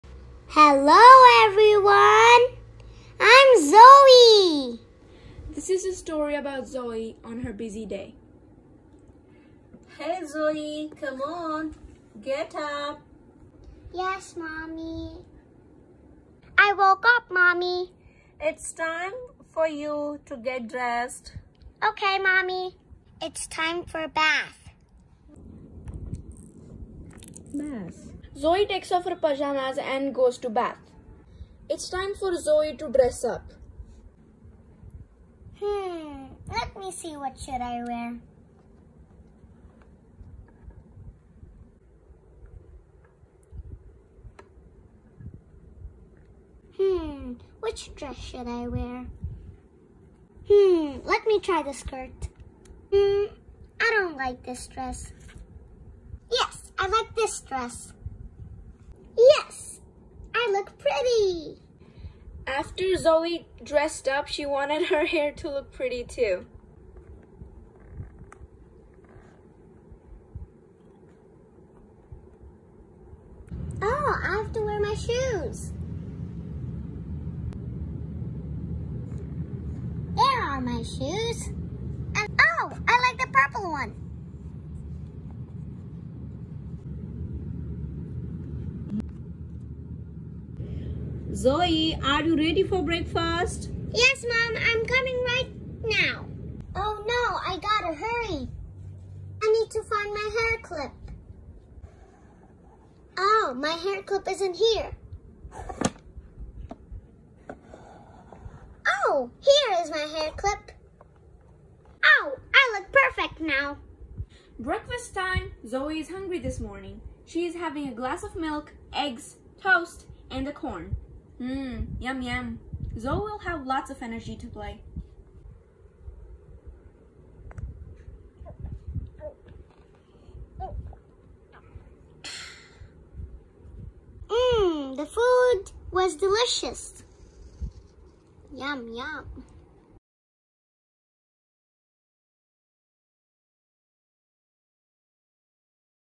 Story for kids-Zoe on her Busy Day Part 1/kids story in English
Here a small story of a girl Zoe
She is so kind and lovable .